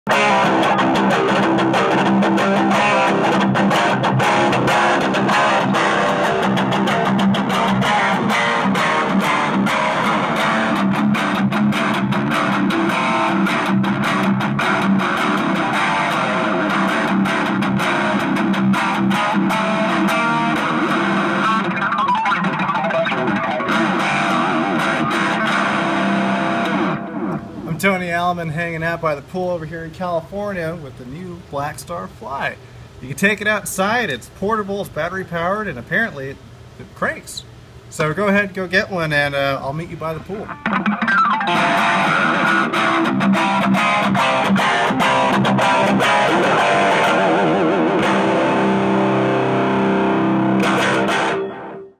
Toni Aleman - Blackstar Fly 3 guitar mini amp demo... On the Fly
Californian guitarist Toni Aleman shreds through the Fly 3... On the Fly